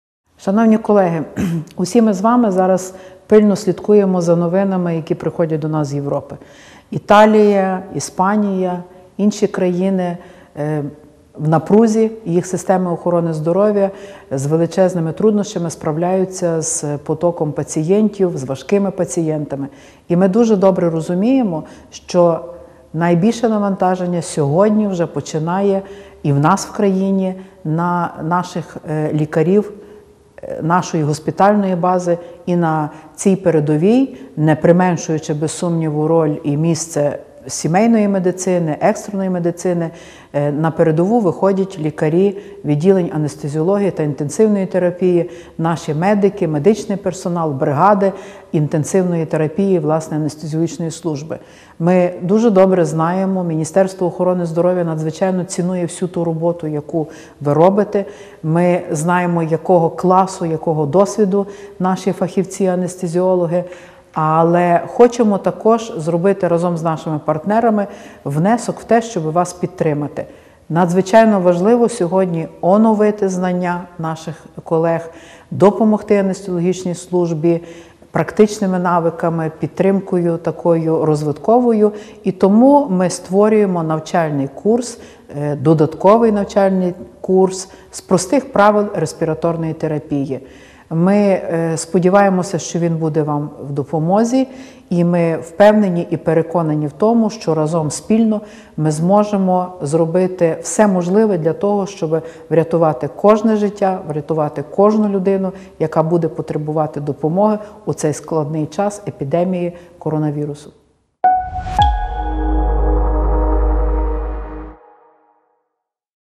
Як працювати з апаратами ШВЛ. Ірина Микичак про «Онлайн-курс із респіраторної терапії»
Фонд Олени Пінчук та Фонд Віктора Пінчука у партнерстві з МОЗ та ЮНІСЕФ розробили онлайн-курс про те, як працювати із апаратами ШВЛ. Він буде корисним лікарям-анестезіологам, реаніматологам та медсестрам, які працюють у відділенні інтенсивної терапії, а також лікарям, медсестрам інших підрозділів медичних закладів і медичним менеджерам, які беруть участь у відповіді на спалах COVID-19.

У відео-блоках зокрема йдеться про будову апаратів штучної вентиляції легень та догляд за ними, первинні налаштування апаратів та адаптацію до пацієнта, тактику проведення вентиляції, важливість зволоження повітряно-дихальної суміші під час ШВЛ тощо.

Викладачами першої частини курсу є Ігор Малиш, завідувач відділення інтенсивної терапії загального профілю Київської міської клінічної лікарні швидкої медичної допомоги, професор кафедри анестезіології та інтенсивної терапії Національної медичної академії післядипломної освіти ім. П.Л. Шупика, анестезіолог вищої категорії, заслужений лікар України, лауреат Державної премії в галузі науки і техніки, доктор медичних наук; Леся Згржебловська, старший ординатор відділення інтенсивної терапії загального профілю Київської міської клінічної лікарні швидкої медичної допомоги, професор кафедри анестезіології та інтенсивної терапії Національної медичної академії післядипломної освіти ім. П.Л. Шупика, доктор медичних наук; Ірина Кондратова, заступник генерального директора Харківської обласної клінічної лікарні — керівник регіонального перинатального центру, кандидат медичних наук, доцент кафедри педіатрії №1 та неонатології Харківського національного медичного університету; Володимир Мітін, експерт з медичного обладнання, офіційний представник компанії-виробника ШВЛ «VYAIRE MEDICAL INC.».